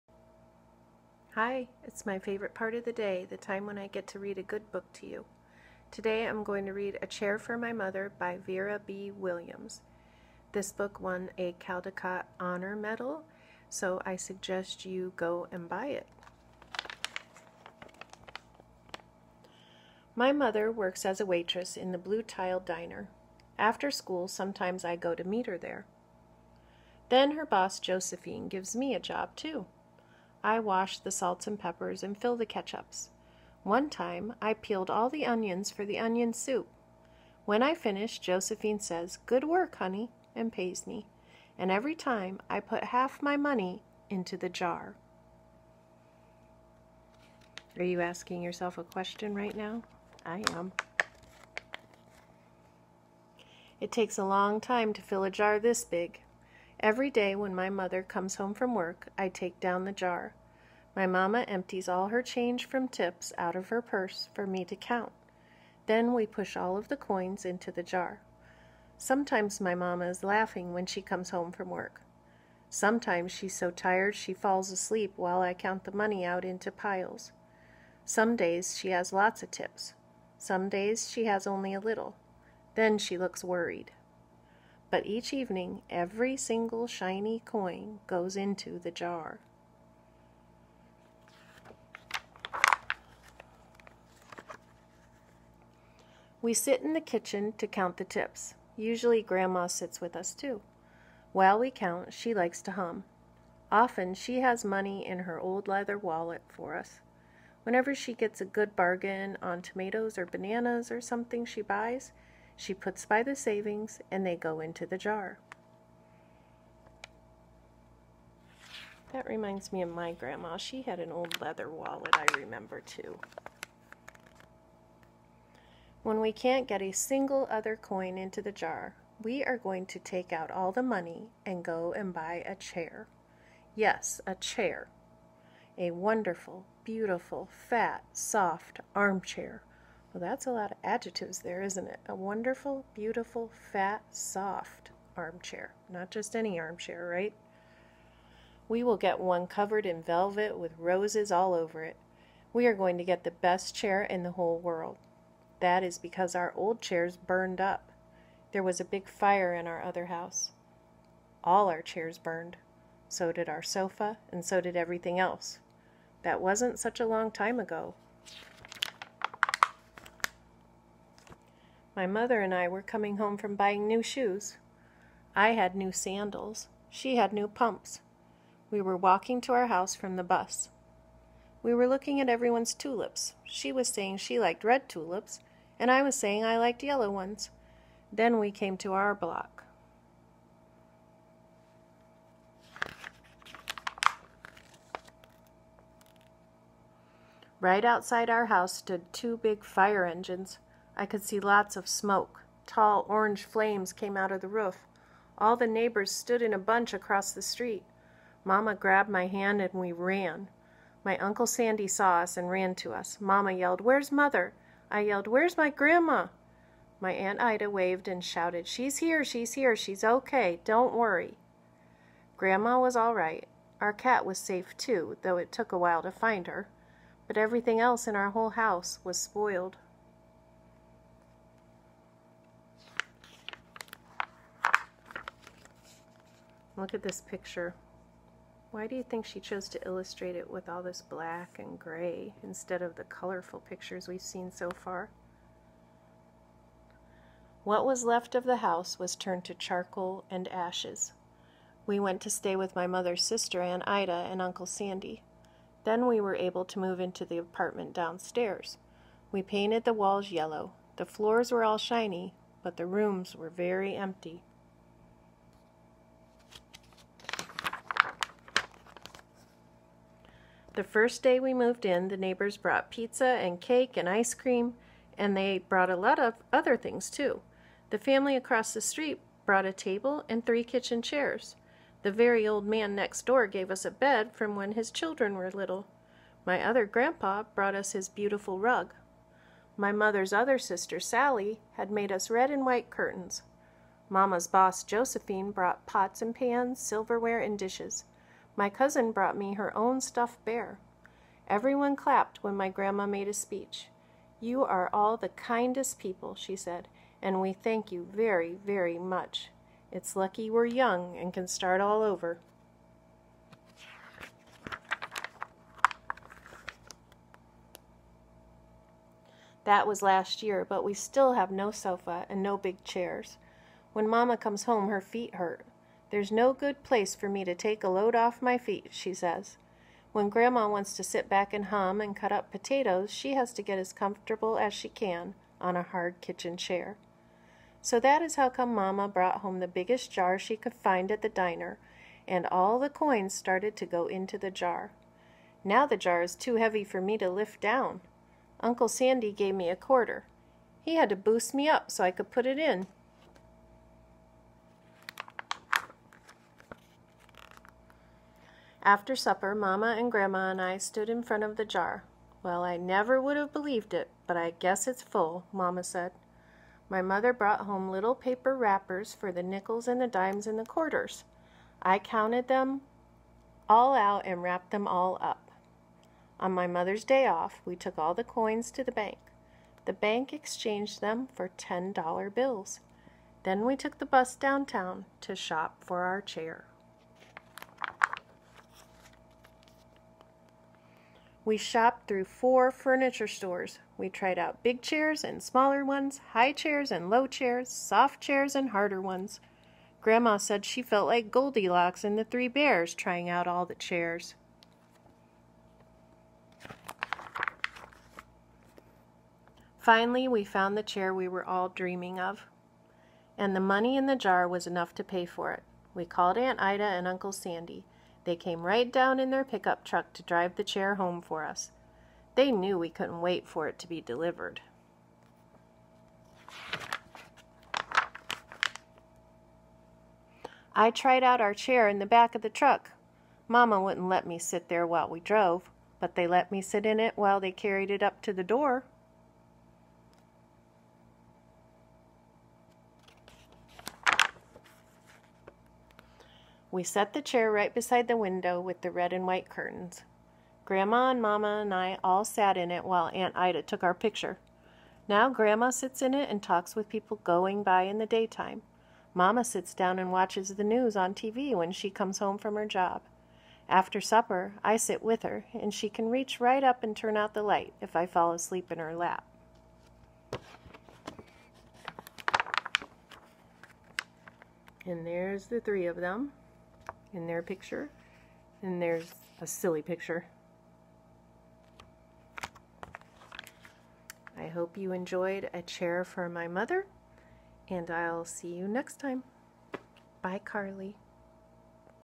A Chair for my Mother
This is a read-aloud of A Chair for my Mother by Vera B. Williams. The AR reading level is 3.4 and the quiz number is 9761. This book beautifully shows the love and strength between family and community.  Because of this it's a great book for teaching theme. Teachers/Parents/Home-Schoolers: This would also be a great book to read before a unit on saving money (for any grade level).  It is also great for teaching inference as much of the story is not directly explained.  It could also be used for teaching sequencing and characteristics. This book is a classic for good reason.  I suggest you take a walk with a family member to check this book out at the library, or buy it to add to your own collection of award winning books.